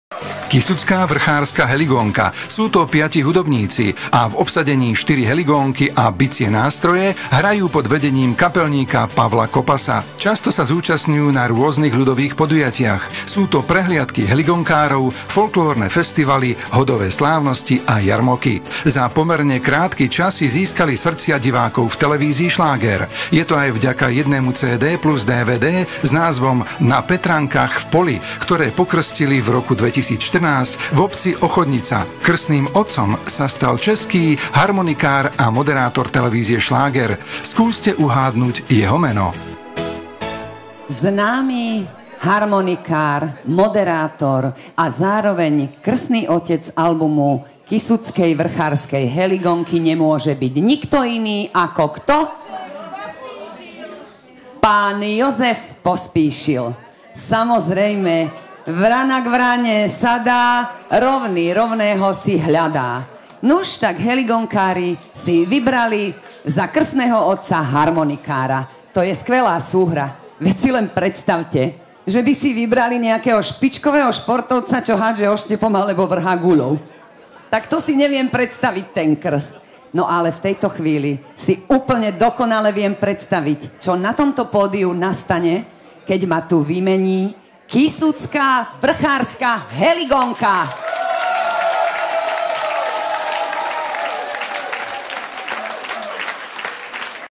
Kysucká vrchárska heligónka
Pesničky z kasína 7 z klubu Blue Note v Novom Meste nad Váhom.
Účinkujú: AT Band, Josef Sochor, Vega, Akropola, Kysucká vrchárska heligónka, Milan Iván